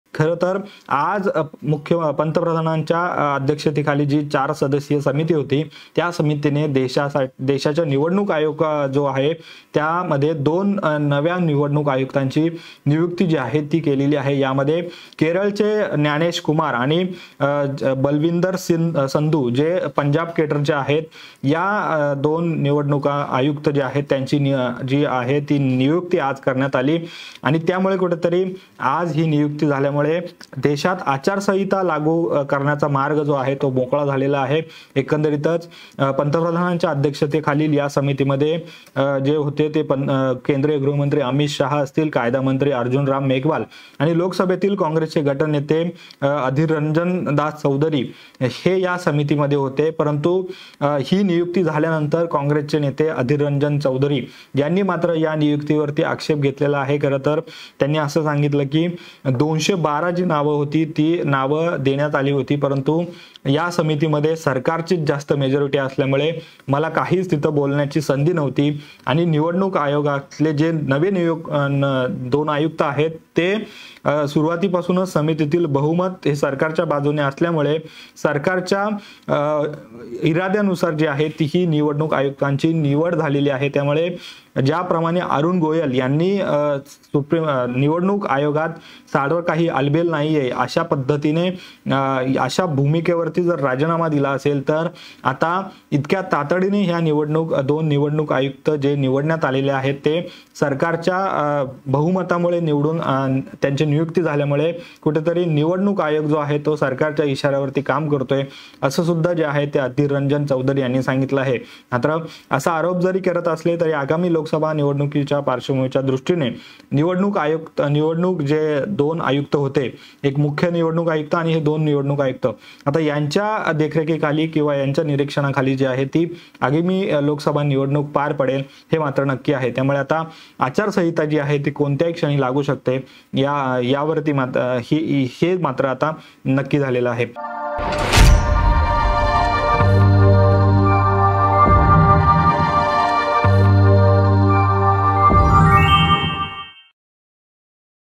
Gyanesh Kumar आणि SS Sandhu यांची देशाचे नवे निवडणूक आयुक्त म्हणून नियुक्ती,Loksabha Election
देशाचे पंतप्रधान नरेंद्र मोदी यांच्या नेतृत्वाखालील 4  सदस्यीय समितीने १४ मार्च रोजी दोन निवडणूक आयुक्तांची नियुक्ती केली आहे ज्ञानेश कुमार आणि सुखबीर संधू (बलविंदर संधू )यांची निवडणूक आयुक्त म्हणून निवड करण्यात आली असे काँग्रेस नेते अधीर रंजन चौधरी माध्यमांना सांगितले.